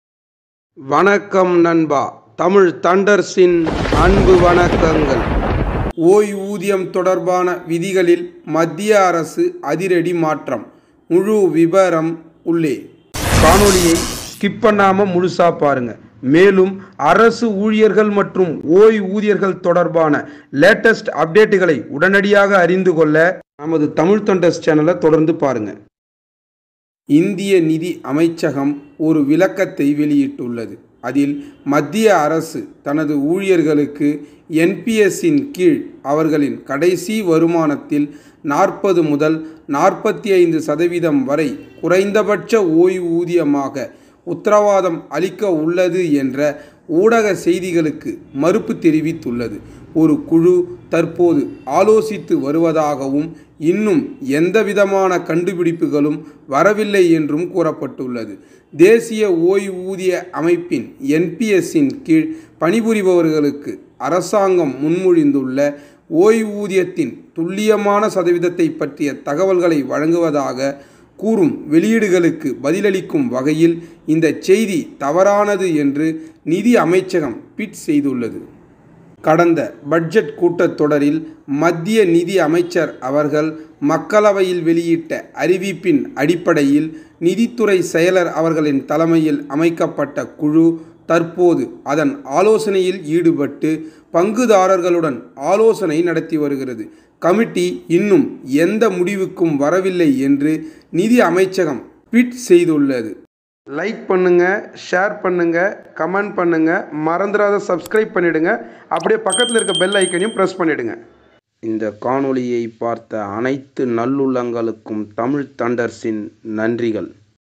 Central Government Pensioners News In Tamil | PENSIONERS NEWS 2023 | PENSION LATEST NEWS IN TAMIL
ஓய்வூதியம் தொடர்பான விதிகளில் அதிரடி மாற்றம் மத்திய அரசு? பதில்

கடந்த பட்ஜெட் கூட்டத்தொடரில் மக்களவையில் மத்திய நிதியமைச்சர் வெளியிட்ட அறிவிப்பின் அடிப்படையில் நிதித்துறை செயலாளர் தலைமையில் அமைக்கப்பட்ட குழு, தற்போது அதன் ஆலோசித்து, பங்குதாரர்களுடன் ஆலோசனையில் ஈடுபட்டு வருகிறது.

தேசிய ஓய்வூதிய அமைப்பின் (NPS) கீழ் பணிபுரிபவர்களுக்கு அரசாங்கம் முன்மொழிந்துள்ள ஓய்வூதியத்தின் துல்லியமான சதவீதத்தைப் பற்றிய தகவல்களை வழங்குவதாகக் கூறும் வெளியீடுகளுக்குப் பதிலளிக்கும் வகையில், "இந்தச் செய்தி தவறானது" என்று நிதி அமைச்சகம் ட்வீட் செய்தது.

The central government will bring drastic changes in the rules related to pension?

Based on the announcement made by the Union Finance Minister in the Lok Sabha in the last budget session, the committee headed by the Finance Secretary is currently deliberating and consulting with the stakeholders.